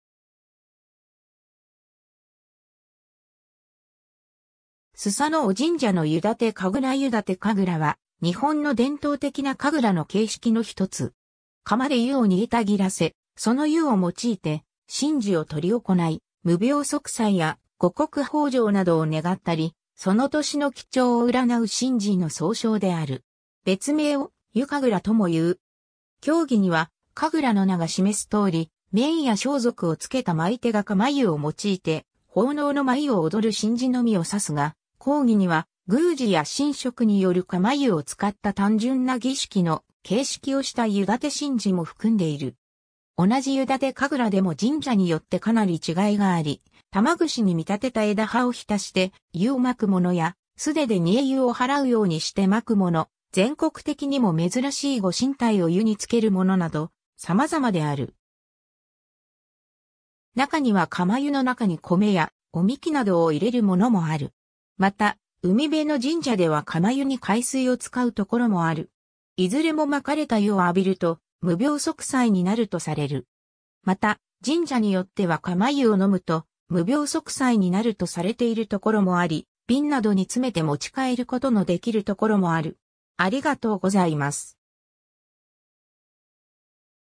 湯立神楽
須佐男神社（兵庫県伊丹市）の湯立神楽（2014年7月23日撮影） 湯立神楽（ゆだてかぐら/ゆたてかぐら）は、日本の伝統的な神楽の形式のひとつ。
 釜で湯を煮えたぎらせ、その湯を用いて神事を執り行い、無病息災や五穀豊穣などを願ったり、その年の吉兆を占う神事の総称である。
別名を「湯神楽（ゆかぐら）」とも言う。
 狭義には“神楽”の名が示すとおり、面や装束をつけた舞い手が釜湯を用いて奉納の舞いを踊る神事のみを指すが、広義には宮司や神職による釜湯を使った単純な儀式の形式をした湯立て神事も含んでいる。
 同じ湯立神楽でも神社によってかなり違いがあり、玉串に見立てた枝葉を浸して湯を撒くもの（神奈川県藤沢市白旗神社など）や、素手で煮え湯を払うようにして撒くもの（長野県飯田市正八幡神社(遠山郷)など）、全国的にも珍しいご神体を湯につけるもの（香川県丸亀市垂水神社など）など様々である。
なかには釜湯の中に米やお神酒などを入れるものもある（京都府京都市城南宮など）。
 また、海辺の神社では釜湯に海水を使うところもある（千葉県横芝光町四社神社など）。
 いずれも撒かれた湯（その飛沫）を浴びると無病息災になるとされる。
 また、神社によっては釜湯を飲むと無病息災になるとされているところもあり、瓶などに詰めて持ち帰ることのできるところもある（長野県天龍村池大神社など）。